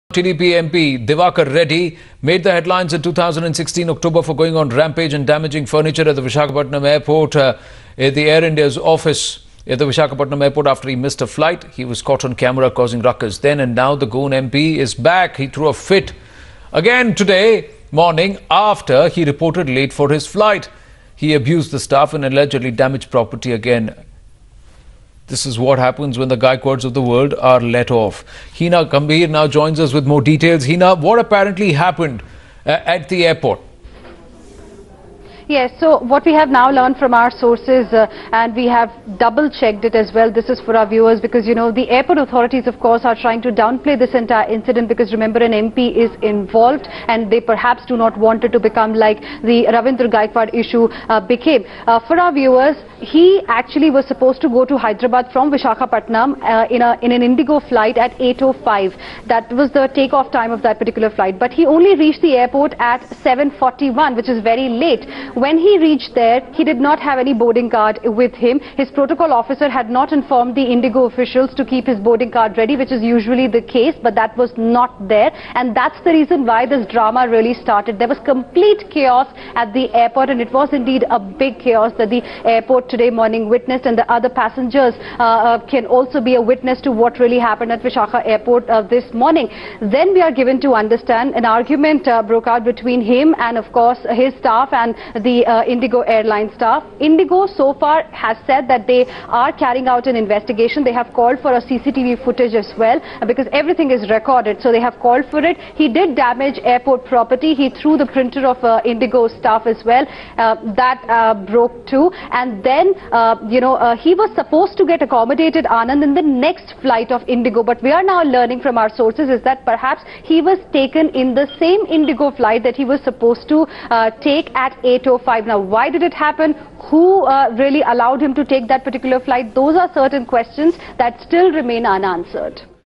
TDP MP JC Diwakar Reddy creates ruckus at Visakhapatnam airport
A Telugu Desam Party (TDP) MP allegedly created a major ruckus at Visakhapatnam airport on Thursday when he reported late for a flight to Hyderabad. And this time like all other VVIPs, he too got away as the airline reportedly accommodated him on the same flight despite the fact that check in for it had closed. To top that, Union aviation minister Ashok Gajapathi Raju - who also belongs to the TDP - was in the VVIP lounge of the airport when the MP was on a rampage.